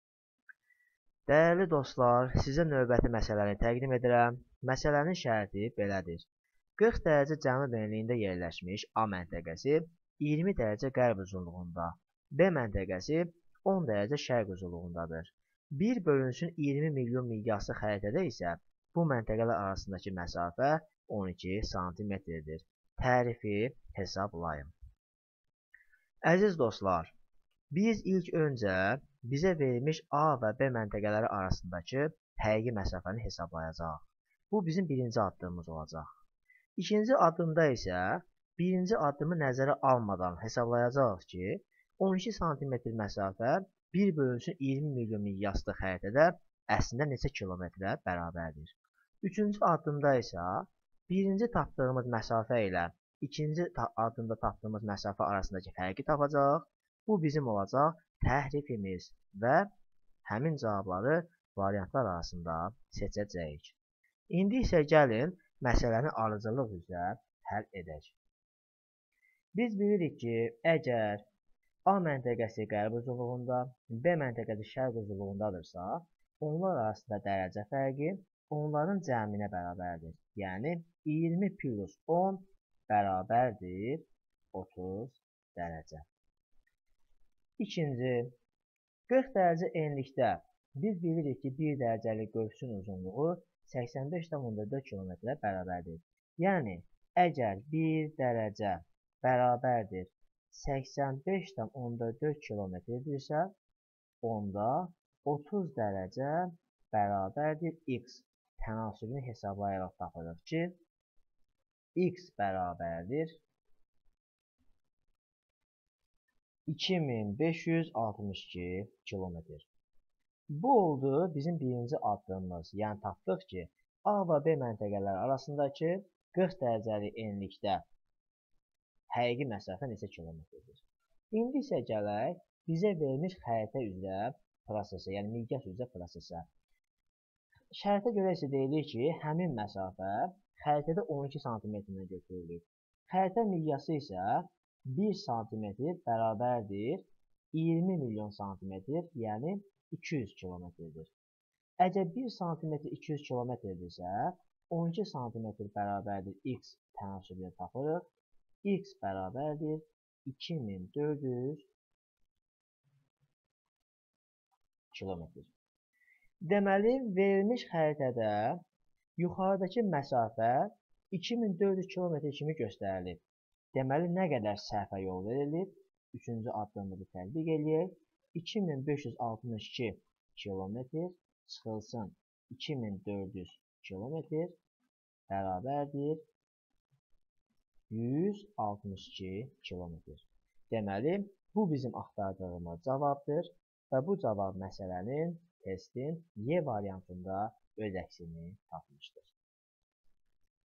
Maraqlı Coğrafiya MSİ məsələsi: TƏHRİFİ hesablayın - Samir m
PRESTİJ-S Mövzu Sınaq İmtahanının Coğrafiya fənni üzrə Maraqlı məsələlərindən biri:

40 dərəcə c.e-də yerləşmiş A məntəqəsi 20 dərəcə q.u-da,  B məntəqəsi 10 dərəcə  ş.u-dadır. 1:20000000 miqyaslı xəritədə isə bu məntəqələr arasında məsafə 12 sm-dir. Təhrifi hesablayın.

A) 348 km           B) 110 km       C) 216 km
D) 854 km           E) 162 km

İzah edir: Samir Salayev (Magistr dərəcəli Coğrafiyaçı)
Tərtib etdi: Rasim Rəhimov (Prestij-S Tədris Mərkəzi)